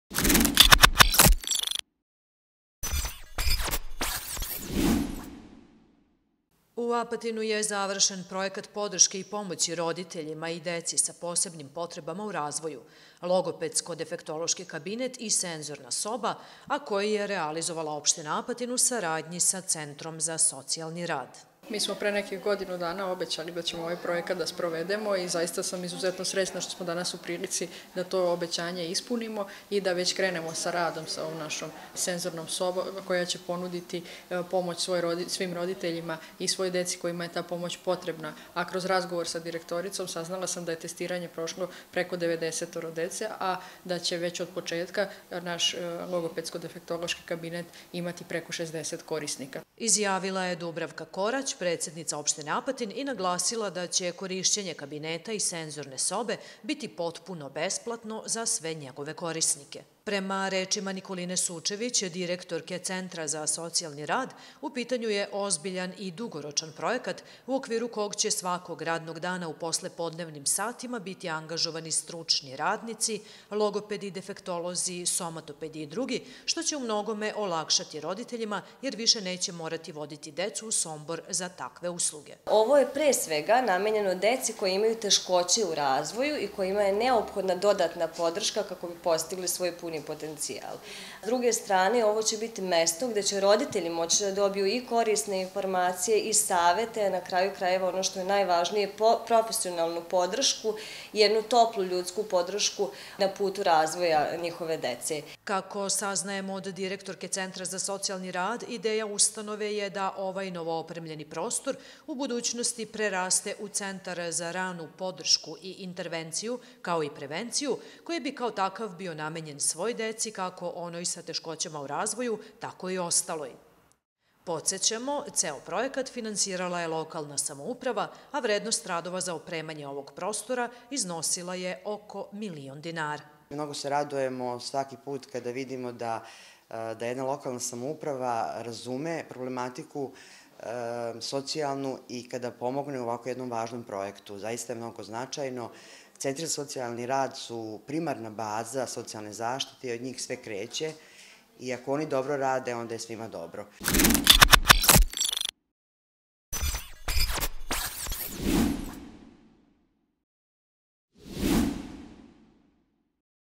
Отворен логопедско-дефектолошки кабинет и сензорна соба
У Апатину је завршен пројекат подршке и помоћи родитељима и деци са посебним потребама у развоју, логопедско-дефектолошки кабинет и сензорна соба, а који је реализовала општина Апатин, у сарадњи са Центром за социјални рад.